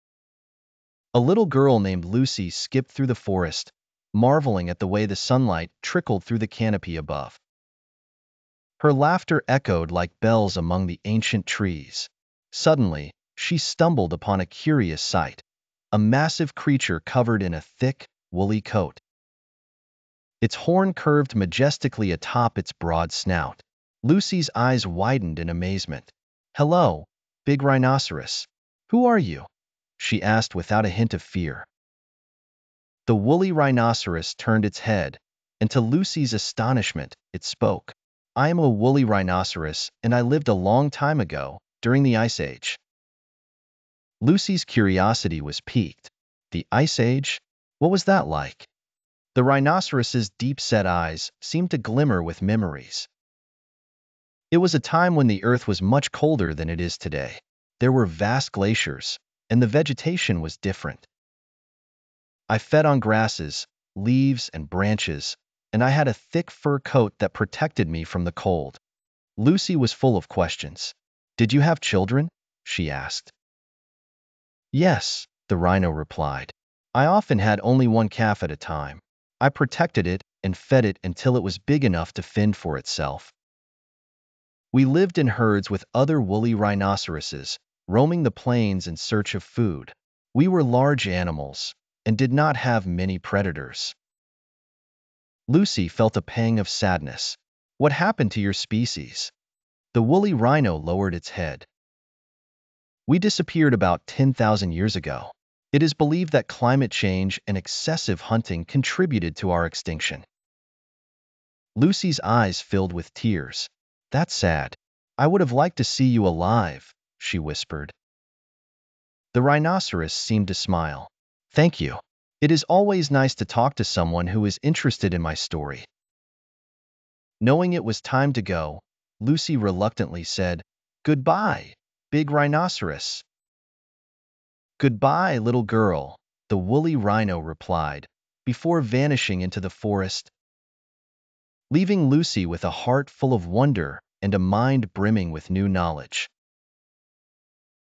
Lucy and the Woolly Rhinoceros
Lucy encounters a woolly rhinoceros from the Ice Age in the forest. The rhino shares its story of survival, extinction, and the impact of climate change, leaving Lucy with a sense of wonder and newfound knowledge.